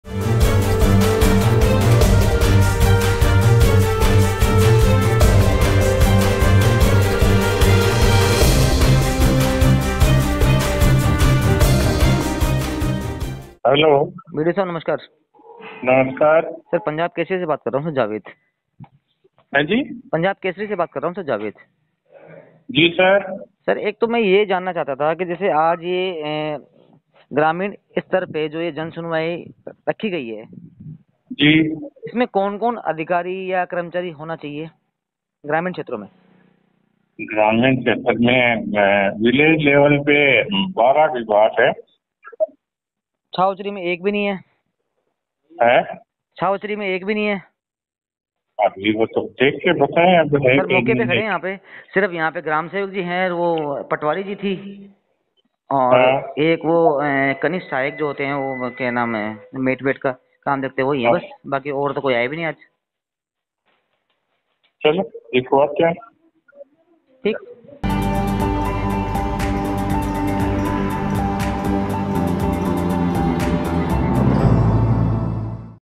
उदयपुरवाटी विधानसभा की ग्राम पंचायत छावसरी मे जनसुनवाई की उड़ी धज्जियां
ग्रामीणों को देखते हुए राज्य सरकार व जिला कलेक्टर ने प्रत्येक महीने के पहले गुरूवार को हर ग्राम पंचायत में जनसुनवाई के आयोजन की शुरुआत की ग्राम पंचायत उदयपुरवाटी विधानसभा की ग्राम पंचायत छावसरी मे ग्रामसेवक कनिष्ठ लिपिक के अलावा जनसुनवाई से सभी नदारद रहे |

Produced By: Punjab Kesari Rajasthan
Script Edited By: rahul
Video Edited By: narendra
Video Journalist: